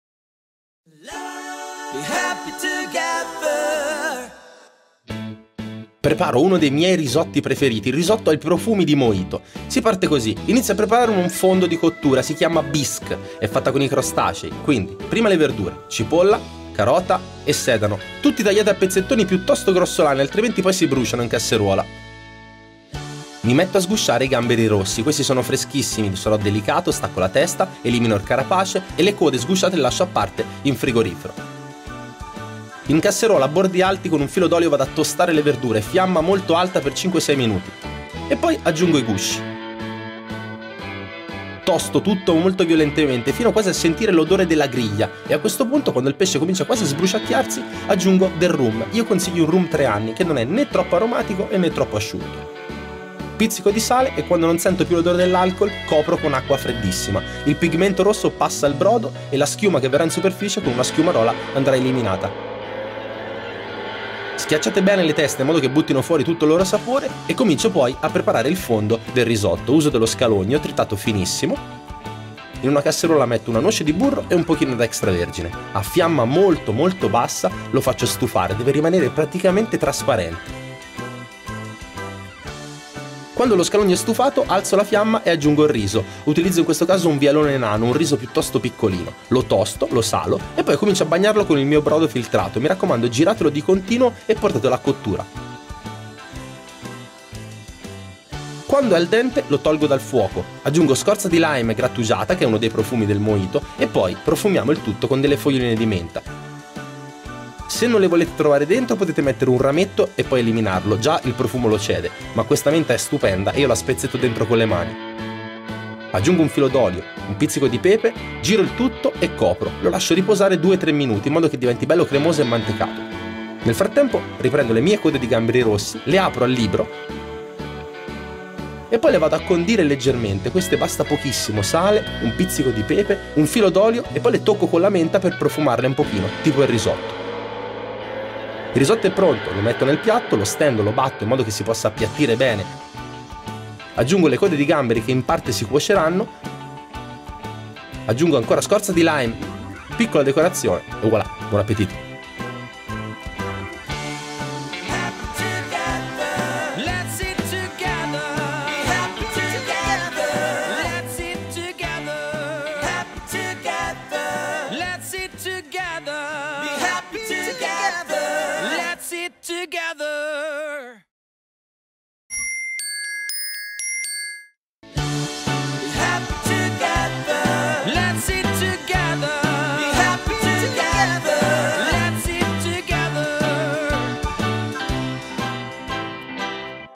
Risotto al Mojito con carpaccio di gamberi rossi
La ricercatezza degli ingredienti e la tecnica di preparazione del tutto innovativa rendono questo piatto davvero unico.
Il risotto è profumato e mantecato con rum, lime e menta: gli ingredienti del famoso cocktail cubano, a cui la ricetta deve il suo nome esotico e accattivante. E i gamberi rossi di Sicilia, pregiati e fragranti, più che un carpaccio diventano un foglio sottilissimo, in cui racchiudere il riso. Una vera invenzione, tutta da copiare.

Vuoi scoprire la ricetta completa di questo e di tanti altri piatti di Simone Rugiati? Vai al canale e scarica il pdf!